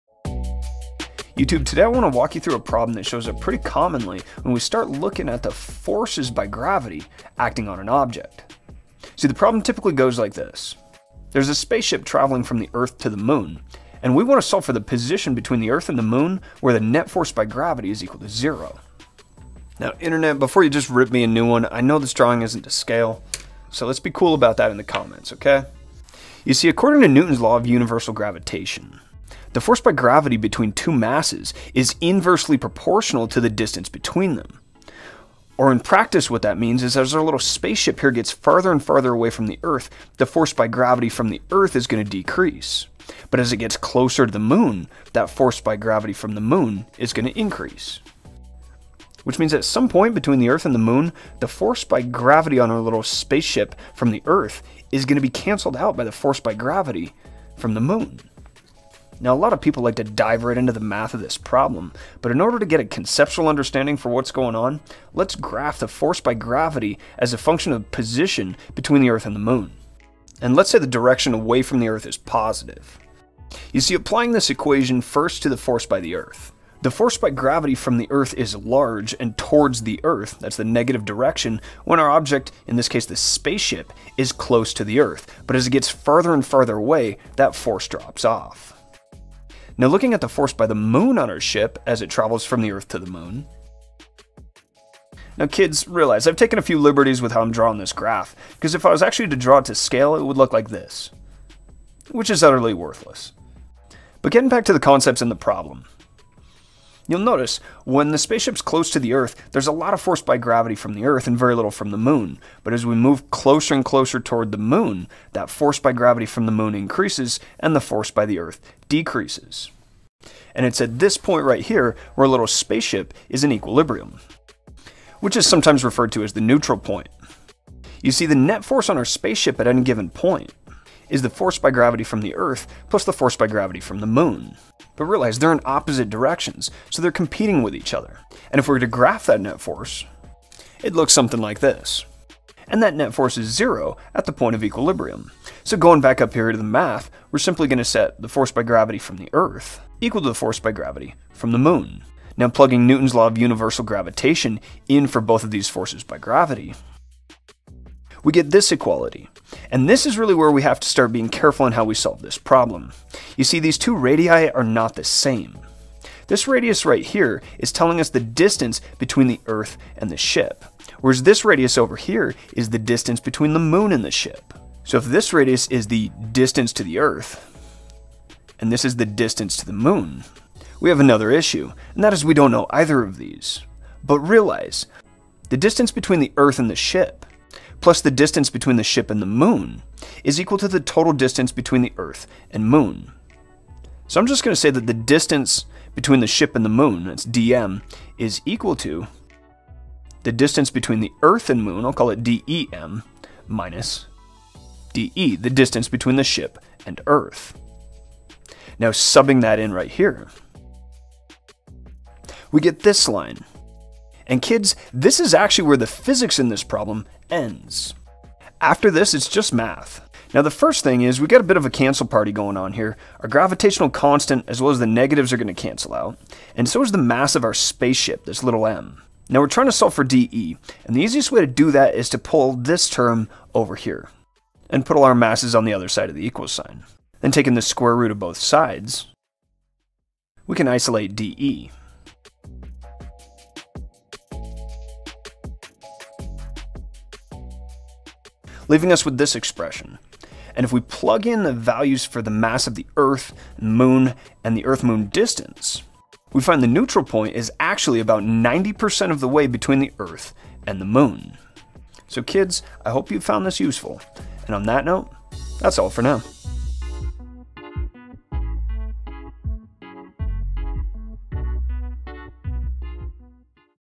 Where Does the Gravitational Force Between Earth and Moon Equal Zero? | The Neutral Point
This is not a Lagrange point.  A Lagrange point requires an object to orbit a central point, thus accelerating.

Calculate the point between the Earth and the Moon where the net force by gravity is zero.  Using Newtons Law of Universal Gravitation, applied to both the Earth and Moon simultaneously, we can see that there is a point, referred to as the Neutral Point or Neutral Line at which the net force is zero.